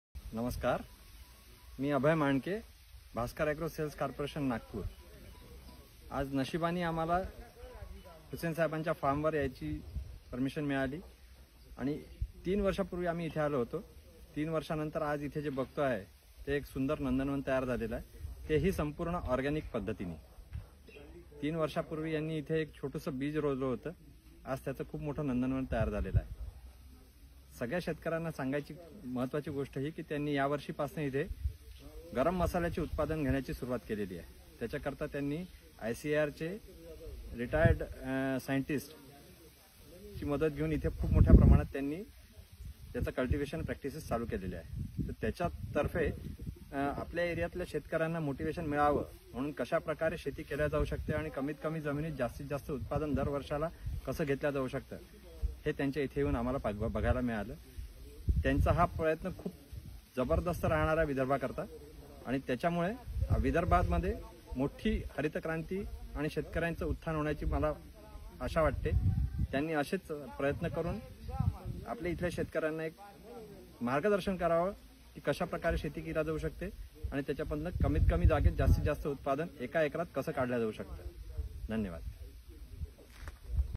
Natural farming,spices in Vidharbha
Abhay Manke of Bhasker Agro visited Asmita Organic Farm, Ramtek.